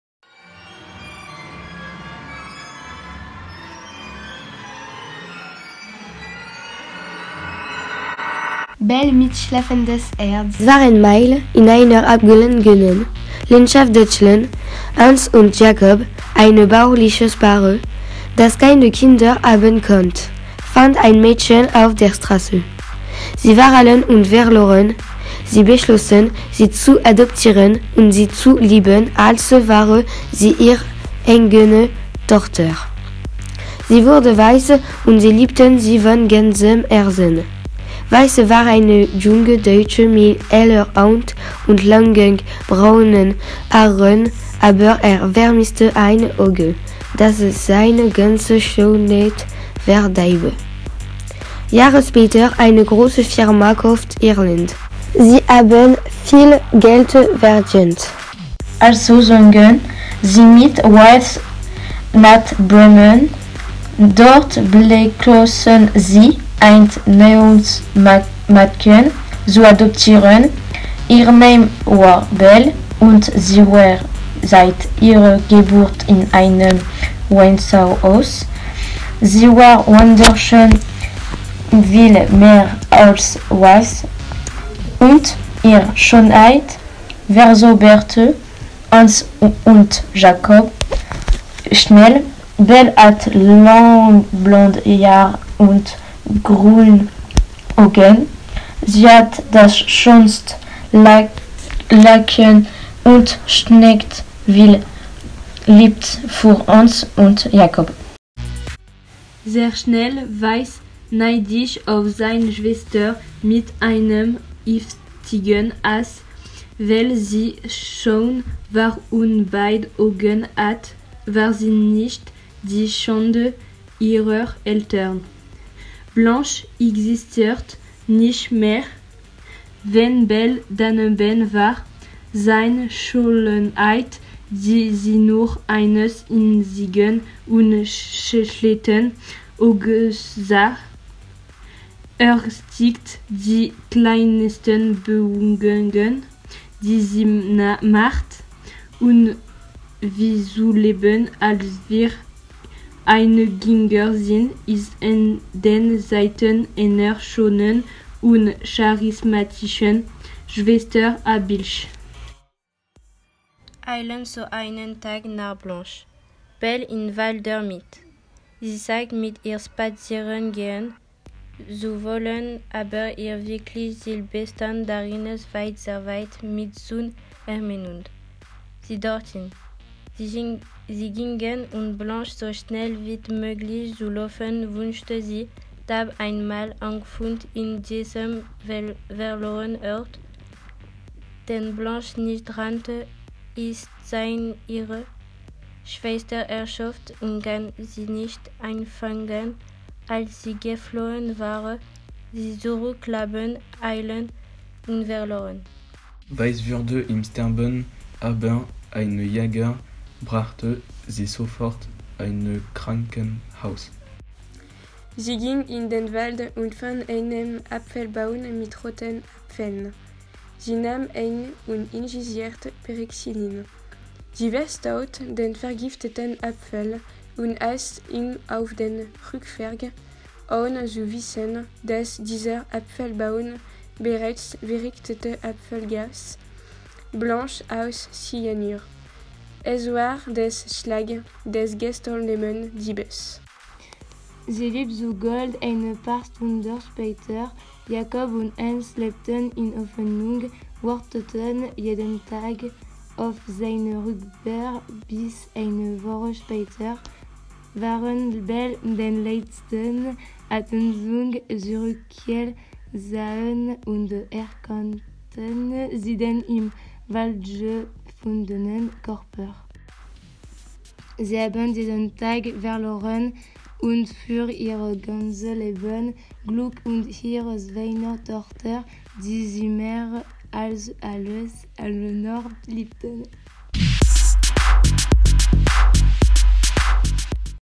Belle mit schlafendes herz - Collège Louis Sandras d'ANIZY LE CHATEAU
Film réalisé dans le cadre de la Première édition du projet « Grimm’Arts » à la Maison de la Culture d’Amiens